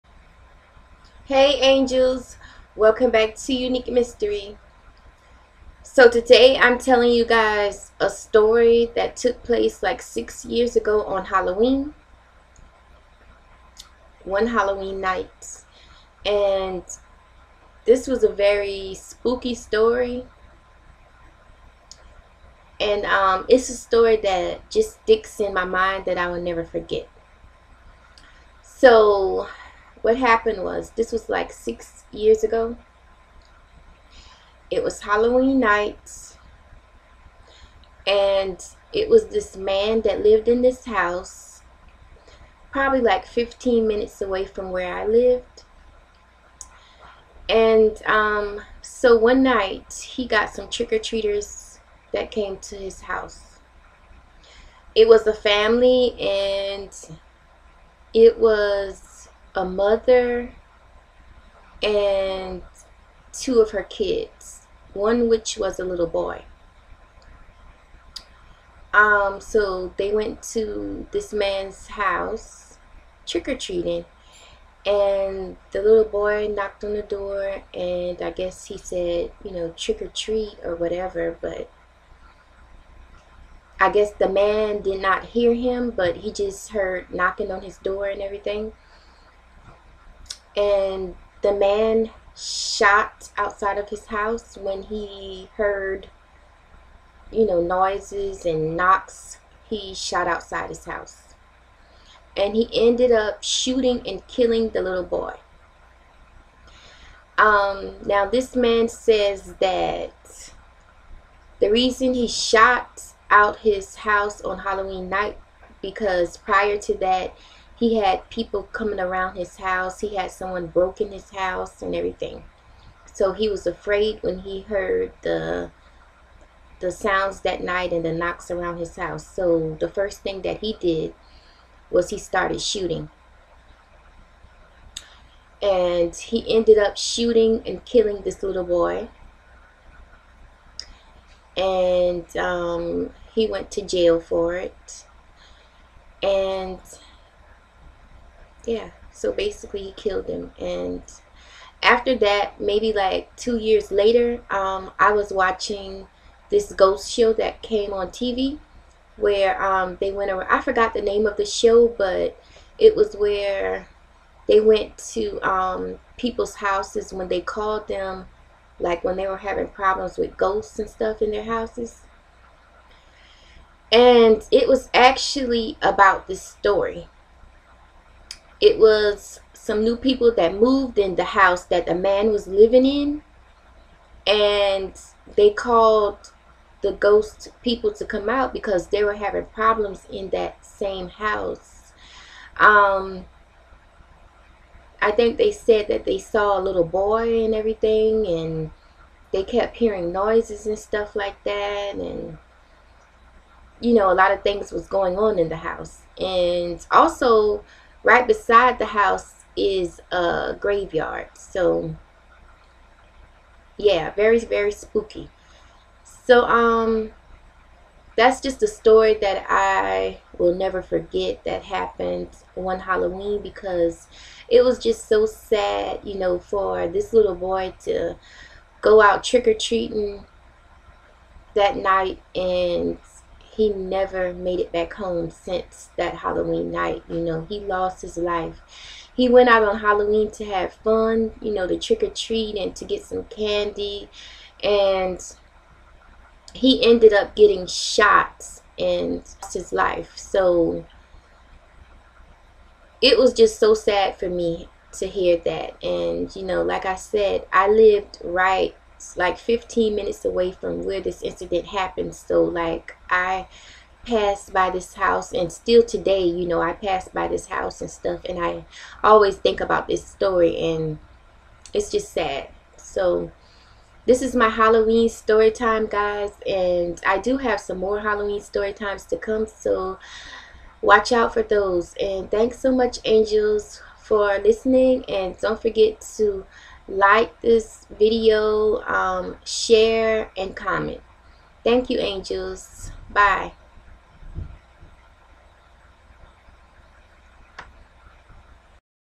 HALLOWEEN STORYTIME
Hey Angels today i'm telling a true Halloween storytime... Don't forget to give this video a big thumbs up...